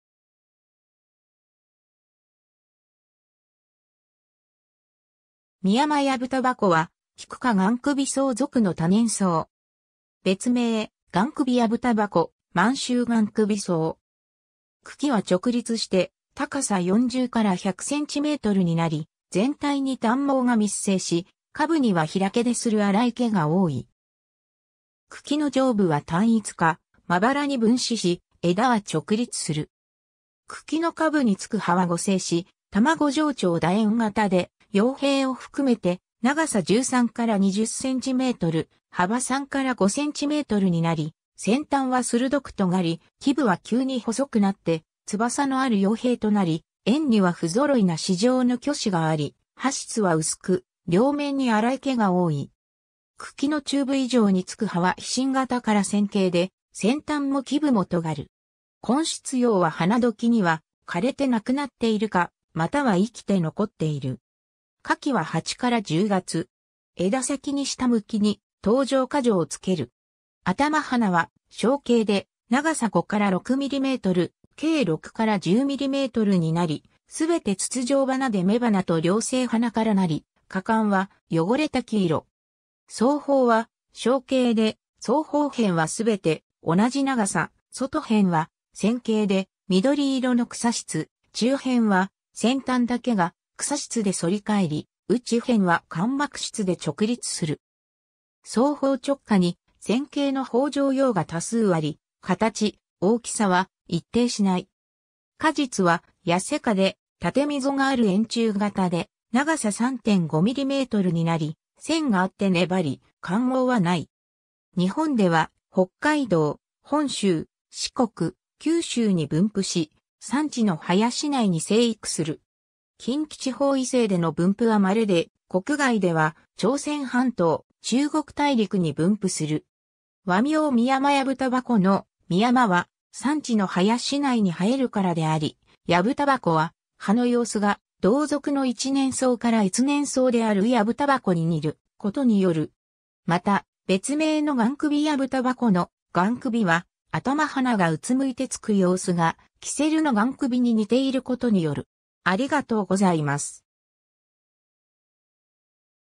ミヤマヤブタバコ
ミヤマヤブタバコ（深山藪煙草、学名：Carpesium triste ）は、キク科ガンクビソウ属の多年草。
別名、ガンクビヤブタバコ（雁首藪煙草）、マンシュウガンクビソウ。
 茎は直立して、高さ40-100cmになり、全体に短毛が密生し、下部には開出する粗い毛が多い。
茎の上部は単一かまばらに分枝し、枝は直立する。
茎の下部につく葉は互生し、卵状長楕円形で、葉柄を含めて長さ13-20cm、幅3-5cmになり、先端は鋭くとがり、基部は急に細くなって翼のある葉柄となり、縁には不ぞろいな歯状の鋸歯があり、葉質は薄く両面に粗い毛が多い。
茎の中部以上につく葉は披針形から線形で、先端も基部もとがる。
根出葉は花時には枯れて無くなっているか、または生きて残っている。
枝先に下向きに頭状花序をつける。
頭花は鐘形で長さ5-6mm、径6-10mmになり、すべて筒状花で雌花と両性花からなり、花冠は汚れた黄色。
総苞は鐘形で、総苞片はすべて同じ長さ、外片は線形で緑色の草質、中片は先端だけが草質で反り返り、内片は乾膜質で直立する。
総苞直下に線形の苞状葉が多数あり、形、大きさは一定しない。
果実は痩果で、縦溝がある円柱形で長さ3.5mmになり、腺があって粘り、冠毛はない。
 日本では、北海道、本州、四国、九州に分布し、山地の林内に生育する。
近畿地方以西での分布はまれで、国外では朝鮮半島、中国大陸に分布する。
 和名ミヤマヤブタバコの「ミヤマ（深山）」は、山地の林内に生えるからであり、「ヤブタバコ（藪煙草）」は、葉のようすが同属の一年草～越年草であるヤブタバコに似ることによる。
また、別名のガンクビヤブタバコの「ガンクビ（雁首）」は、頭花がうつむいてつくようすがキセルの雁首に似ていることによる。